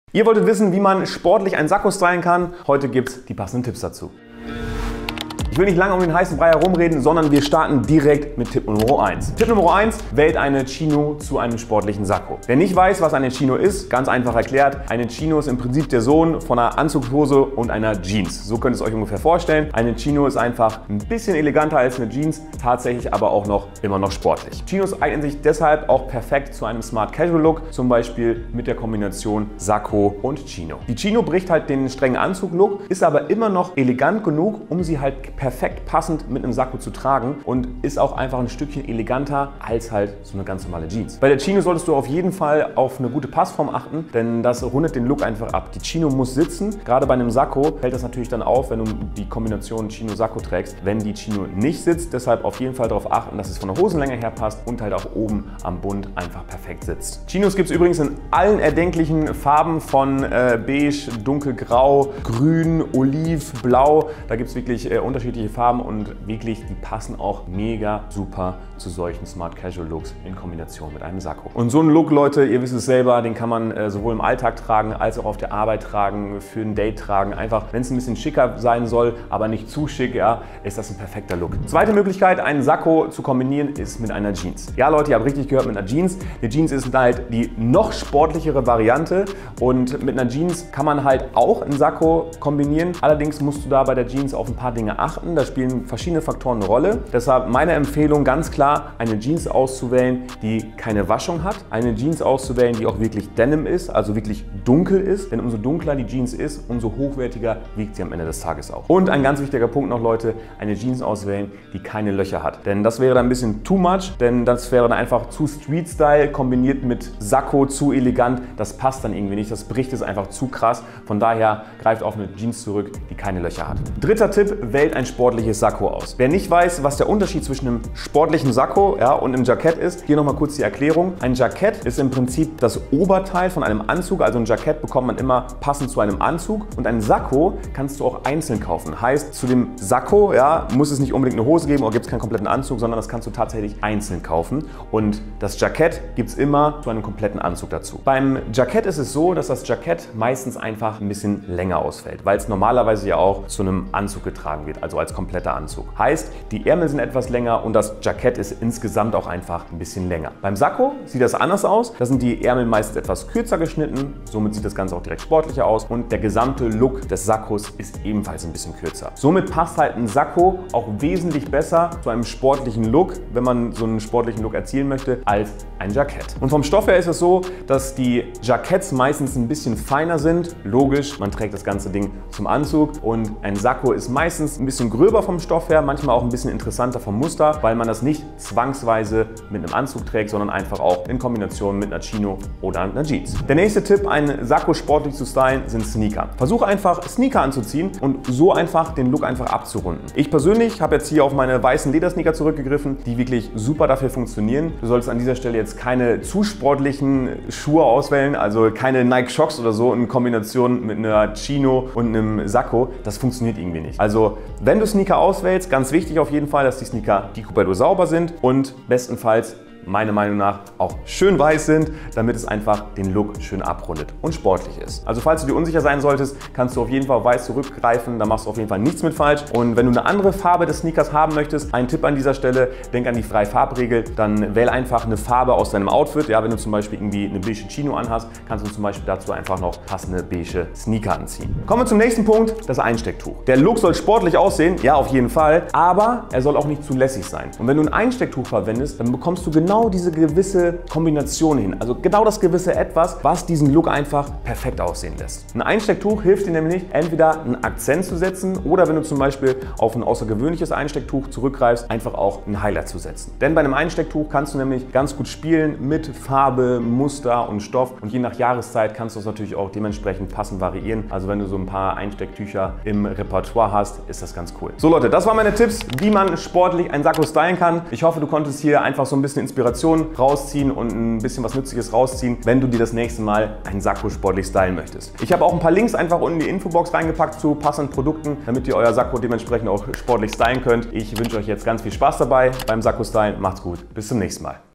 Sakko sportlich stylen ● Sytlingtipps für Männer
Heute gibt's mal wieder Stylingtipps für Männer. Diesmal sind die Stylingtipps speziell für ein sportliches Sakko. Ihr wolltet nämlich wissen, wie man ein Sakko stylen kann bzw. wie man ein Sakko sportlich stylt und in diesem Video gibt es die passendes Styling Tipps dazu. Wir Männer tun uns beim Outfits kombinieren meist einfach ein wenig schwerer, was aber auch kein Problem, solange ihr meinen YouTube Kanal abonniert habt und somit regelmäßig mit passenden Styling Tipps für Männer und Styling Tipps für Jungs versorgt werdet ;) Nein Spaß beiseite... Ich zeige euch in diesem Video verschiedene Möglichkeiten, wie man als Mann ein Sakko sportlich stylen bzw. welche Kleidung man gut mit einem sportlichen Sakko kombinieren kann. Man kann ein Sakko mit Jeans stylen, aber auch mit einer Chino oder mit weißen Ledersneaker... Es gibt verschiedene Möglichkeiten ich hoffe, dass ich dir mit diesem Video einfach ein wenig Inspiration bieten kann, wenn du dir das nächste Mal ein Sakko kombinierst. Wenn ihr Lust auf weitere Styling Tipps für Männer habt oder mehr Fragen zu den Styling Tipps beantwortet haben wollt, schreibt es einfach in die Kommentare :)